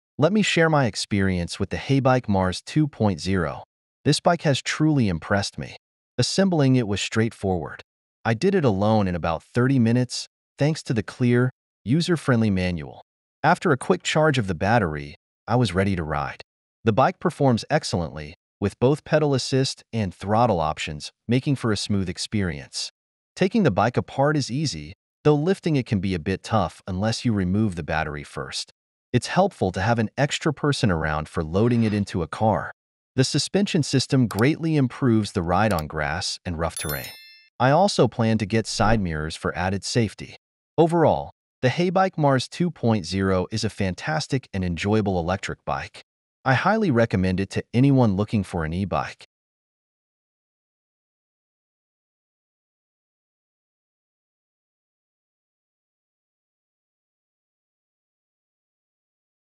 Heybike Mars 2.0: Effortless Assembly & Top Performance
Heybike Mars 2.0

Discover the Heybike Mars 2.0—a game-changing electric bike that's easy to assemble and delivers exceptional performance. With a user-friendly setup, smooth ride options, and effective suspension, it's perfect for both casual and off-road adventures. Plus, find out why this e-bike is highly recommended for anyone in the market!

00:00 Effortless Assembly and Impressive Performance
00:22 Easy Breakdown and Enhanced Ride Quality
00:40 Recommended Upgrades and Overall Recommendation